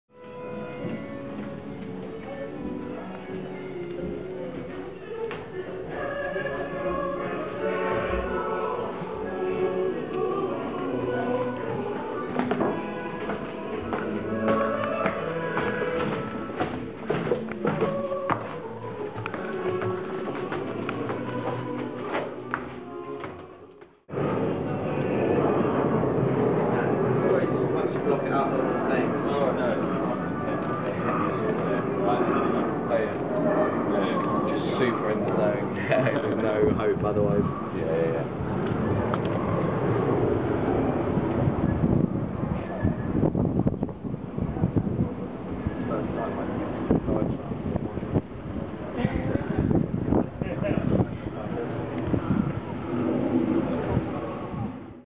Entrance to the top of Churchline in Lublin at Urban Highline Festival
Interesting entrance way to the 40m high Churchline in Lublin, Poland, at Urban Highline Festival